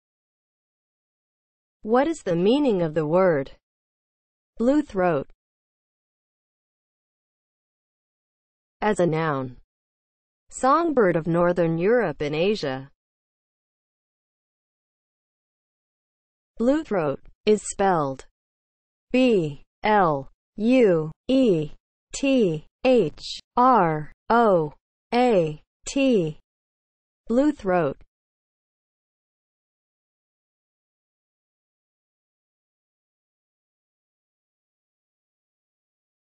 What is the meaning of the word BLUETHROAT?
Definition and spelling of the word BLUETHROAT.

(00:00) Intro
(00:07) As a noun
(00:14) Spelling

As a noun (00:07):
• songbird of northern Europe and Asia